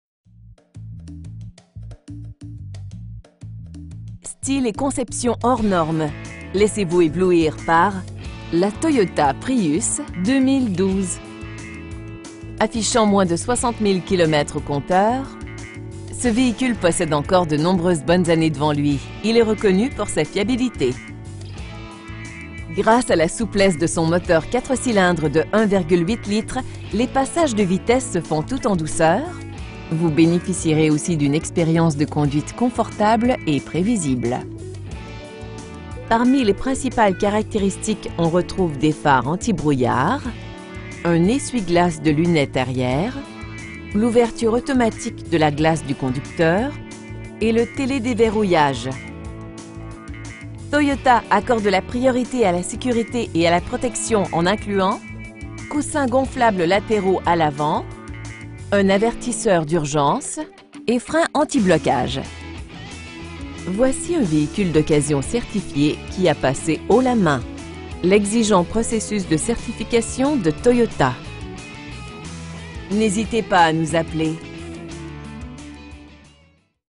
2012 Toyota Prius Hybride A/C+VITRES+PORTES+CAMERA
Toyota/Scion Pr��sident
1920, Ste-Cath��rine Ouest
in Montr��al, QC H3H 1M4

Style et conception hors-normes, laissez-vous ��blouir par la Toyota Prius 2012. Affichant moins de 60 000 km au compteur, ce v��hicule poss��de encore de nombreuses bonnes ann��es devant lui, il est reconnu pour sa fiabilit��. Gr��ce �� la souplesse de son moteur 4 cylindres de 1,8 litres, les passages de vitesses se font tout en douceur, vous b��n��ficierez aussi d'une exp��rience de conduite confortable et pr��visible. Parmi les principales caract��ristiques on retrouve des phares antibrouillard, un essuie-glace de lunette arri��re, l'ouverture automatique de la glace du conducteur, un ordinateur de bord, une banquette arri��re �� dossiers rabattables s��par��s, climatiseur, console de plafond, et le t��l��d��verrouillage. L'habitacle propose de nombreuses solutions de rangement qui sont le reflet d'un souci ��lev�� du d��tail. L�� o�� il fait plus froid, les occupants appr��cieront �� leur juste valeur les si��ges chauffants et le dispositif de climatisation contr��l��e. Ses ��l��gantes roues en alliage soulignent �� la fois son raffinement et son allure sportive. Toyota accorde la priorit�� �� la s��curit�� et la protection en incluant: rideaux gonflables pour prot��ger la t��te, coussins gonflables lat��raux �� l'avant, syst��me antipatinage, syst��me d'assistance au freinage, appuis-t��te avant anti-coup de fouet, un avertisseur d'urgence, et freins antiblocage. Pour plus de s��curit��, l'entra��nement est coupl�� �� un syst��me du contr��le dynamique de la stabilit��. Voici un v��hicule d'occasion certifi�� qui a pass�� haut la main l'exigent processus de certification de Toyota. Notre objectif: offrir les meilleurs prix et le meilleur service �� chacun de nos clients. N'h��sitez pas �� nous appeler.